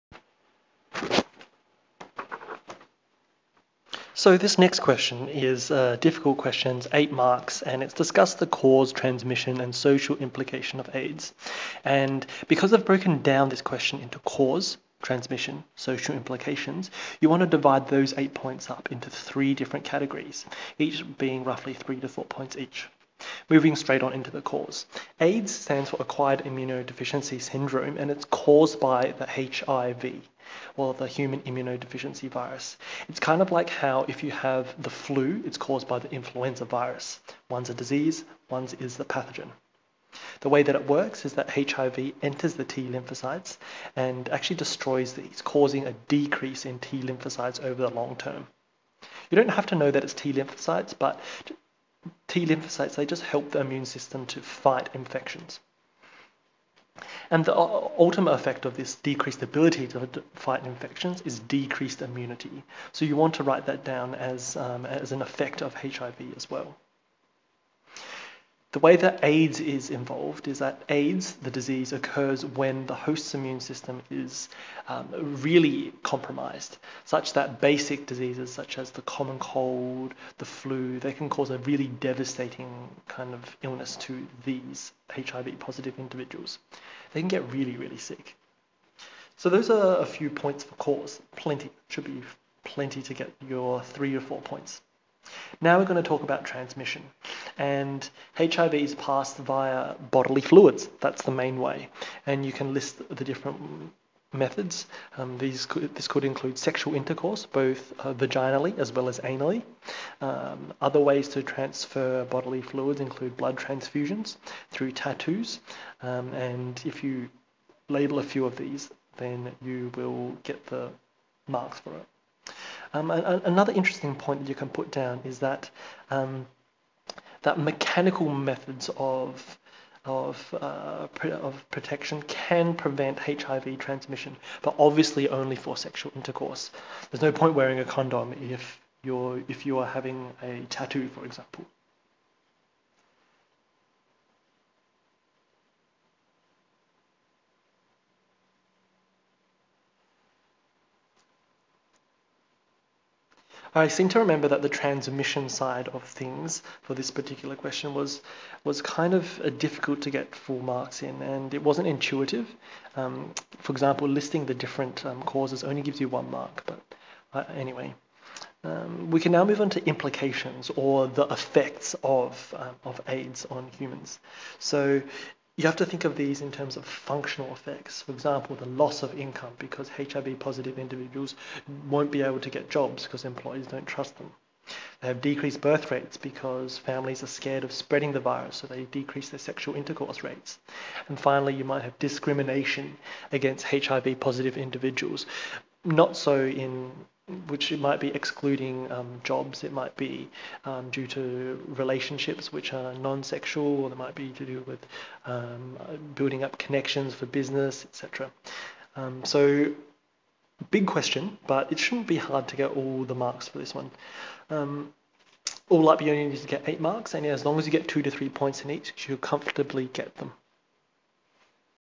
IB SL Biology Past Paper 2 Questions - Question 10 [HIV & AIDS]
Discuss the cause, transmission and social implications of AIDS.

AIDS is increasing even in well developed countries, so it's not surprising the IB want you to know about this devastating disease in some detail.

Whenever you get long questions worth between 7 to 9 marks, you want to break down the question into different segments. For this question, you can write 2-3 marks about the cause, transmission and then social implications which will net you over the 8 marks required.

Don't be put off if the marks on the mark scheme are really random, they can sometimes happen like that :(